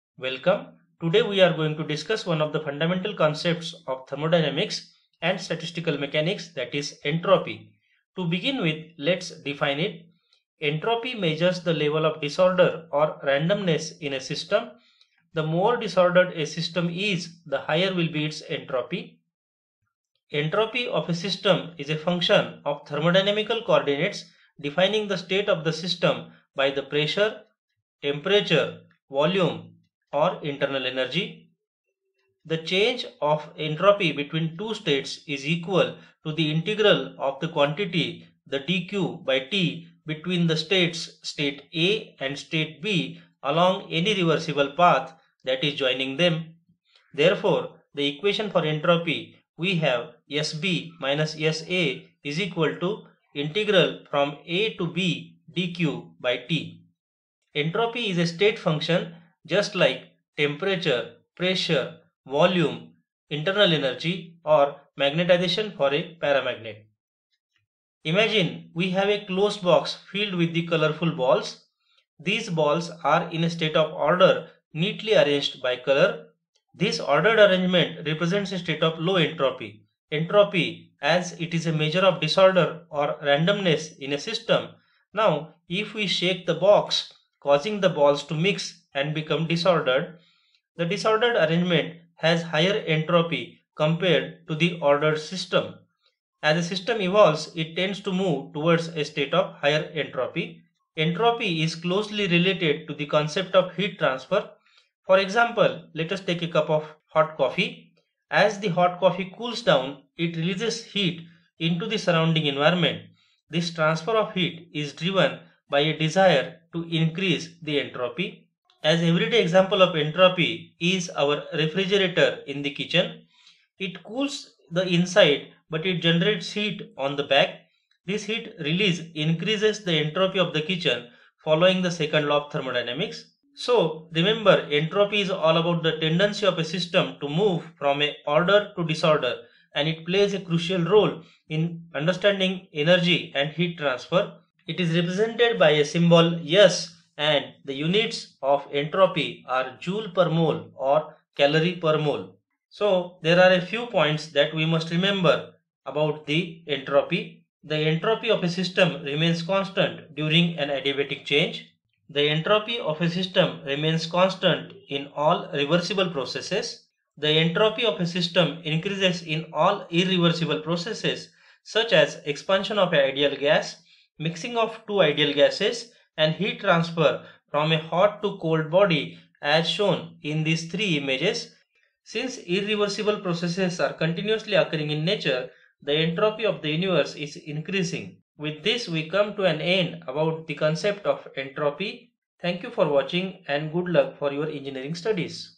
📘Entropy | simple explanation | Demystifying Entropy
🔍 What You'll Discover:
Defining Entropy: Understand the concept of entropy and its connection to the disorder or randomness of a system.
Entropy in Different Systems: Explore how entropy manifests in various physical systems, from gases to solids, and its impact on their behavior.
Entropy is a cornerstone in thermodynamics, and grasping its principles is essential for comprehending the workings of heat, energy, and processes in engineering. This video simplifies complex ideas, making them accessible to first-year students and anyone intrigued by the wonders of thermodynamics.
This video is tailored for first-year engineering students, but it's also a valuable resource for anyone keen on gaining insights into the fundamental principles that govern our physical world. No prior knowledge of thermodynamics is required.